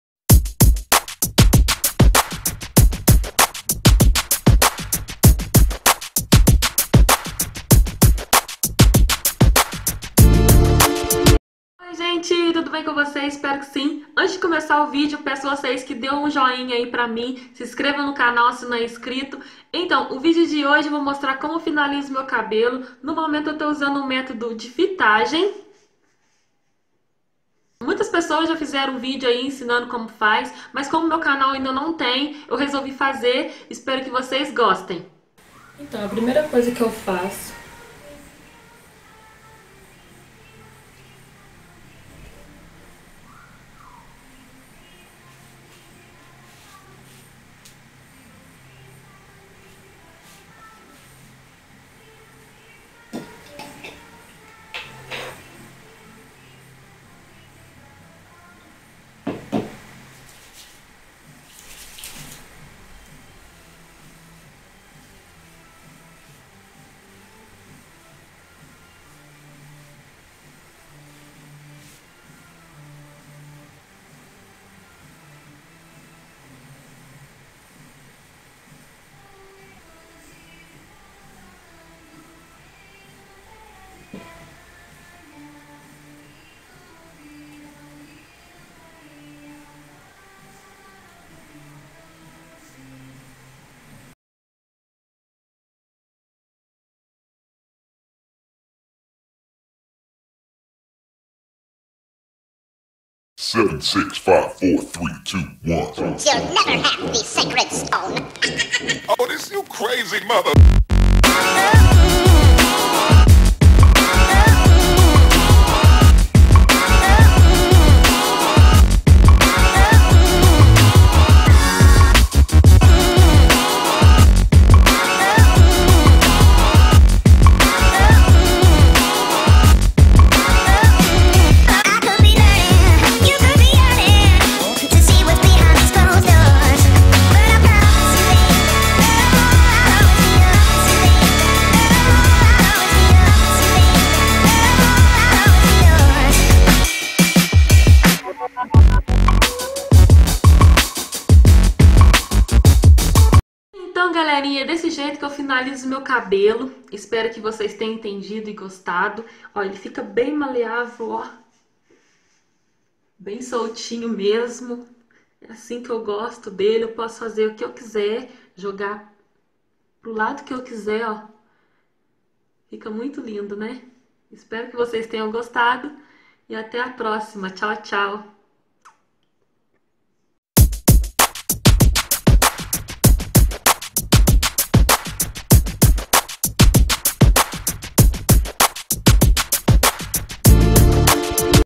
COMO FINALIZO O MEU CABELO CRESPO - POR NEYMARA LIMA
Nesse vídeo irei mostrar como finalizo meu cabelo crespo.
Beijos.